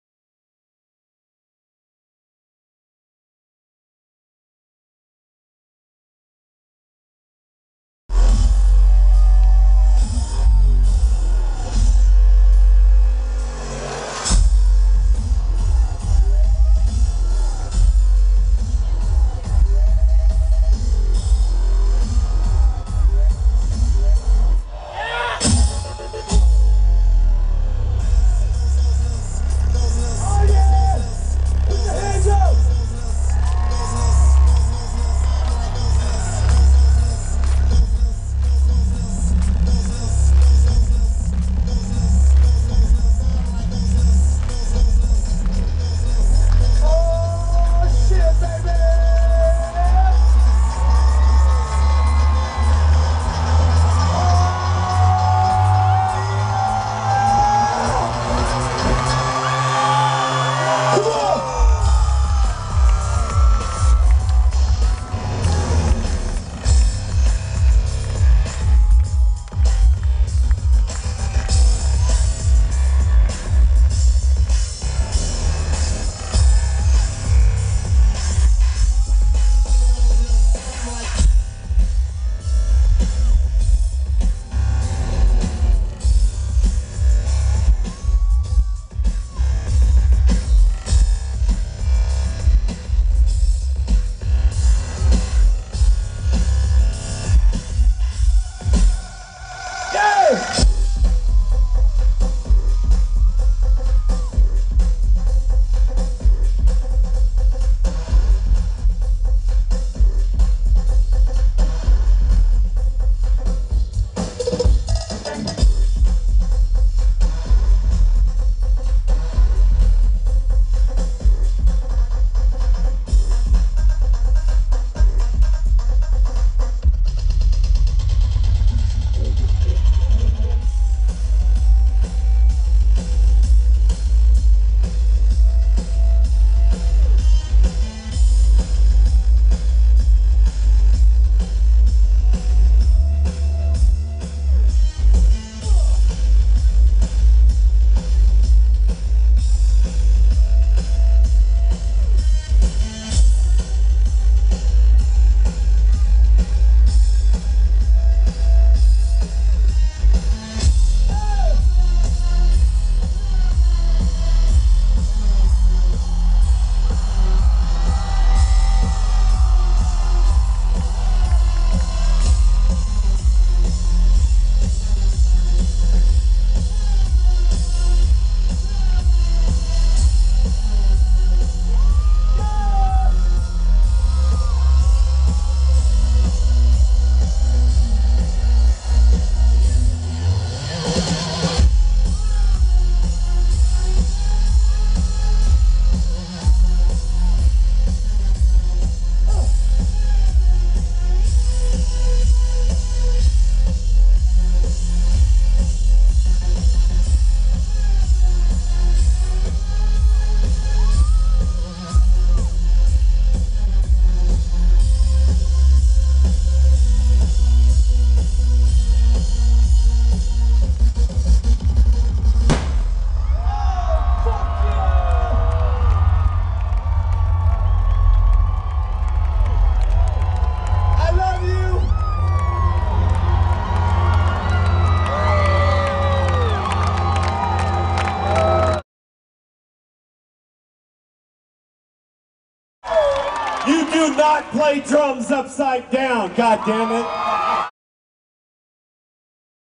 Tommy Lee's 360 Drum Solo!
Live in Hershey on their 2011 tour. Thanks Tommy Lee for tweeting this video!